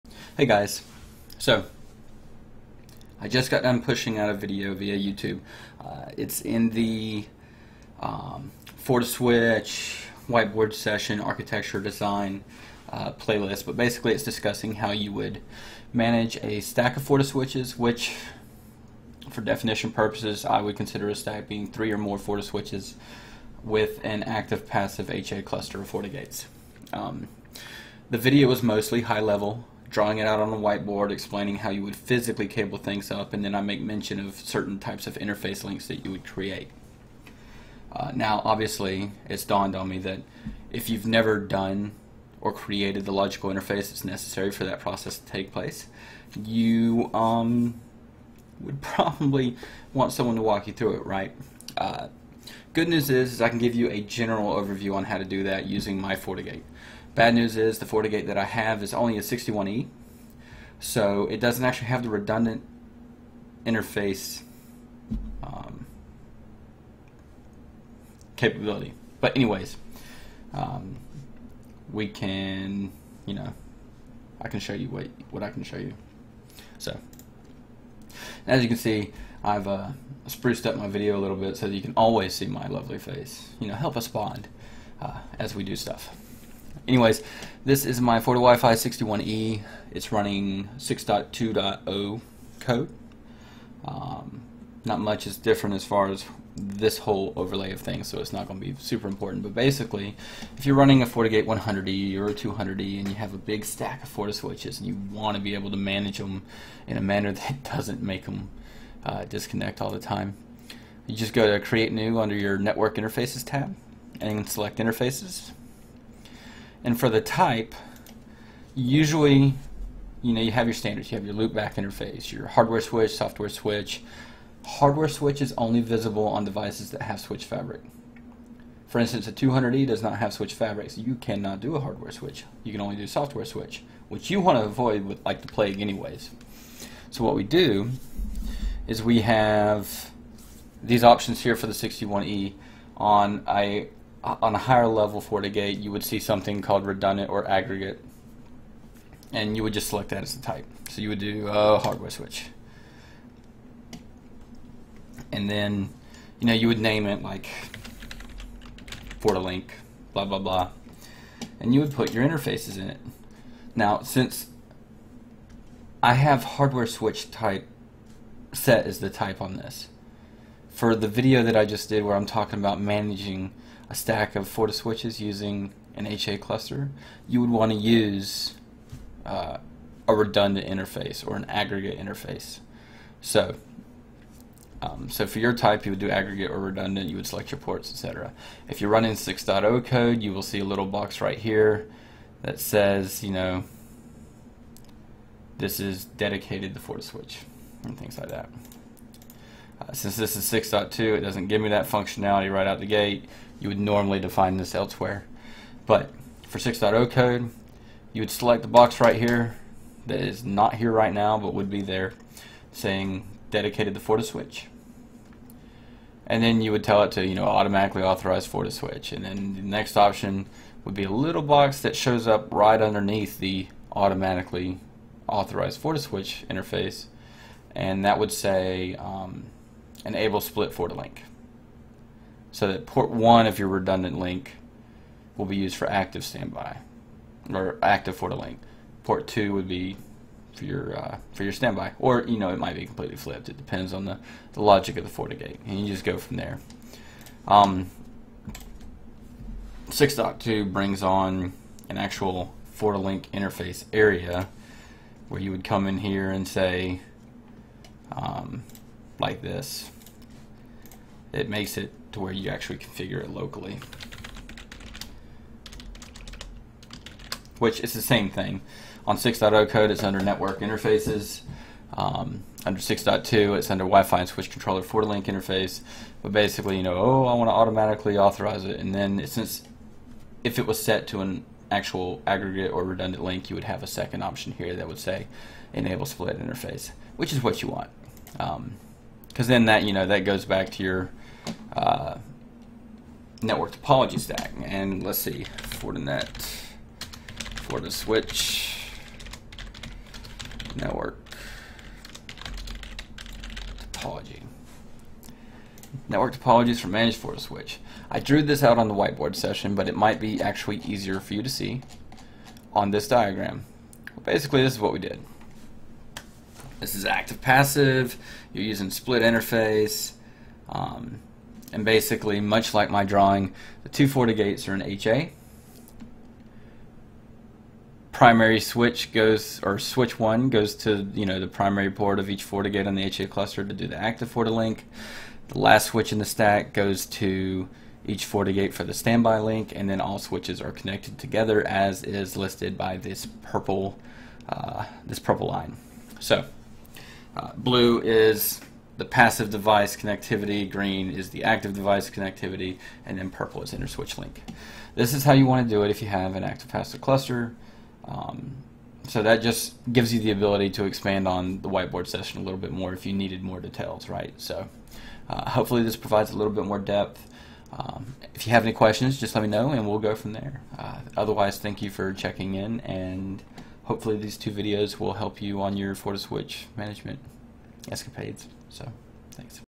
HA FortiGate Managing FortiSwitch Stack PART 2
From the FortiGate perspective HA management of FortiSwitch as well as the Fortinet Diagram explaining the cabling. This may be clearer than my white board session.